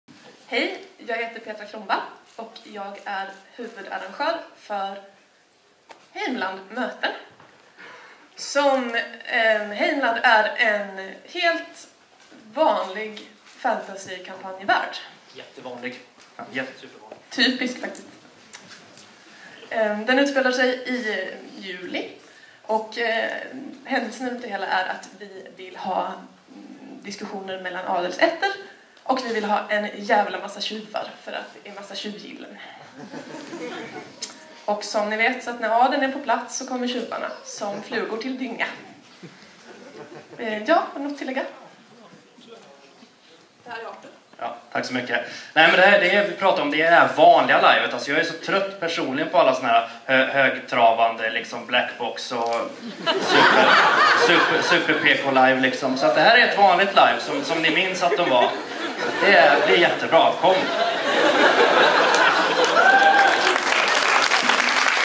Heimland: Möten
I sommar kommer Pegas lajvförening arrangera ett fantasylajv i kampanjen Heimland. Fokuset ligger på interagerande mellan adelsätter (det ska delas upp jordlotter mellan sig, förbund ska skapas och säkerligen kommer en och annan konflikt blossa upp) samt på ett tjuvgille som ska tävla i att komma över bäst och mest värdefulla föremål, gärna framför näsan på adeln.
Inte coolast men ett gött lajv.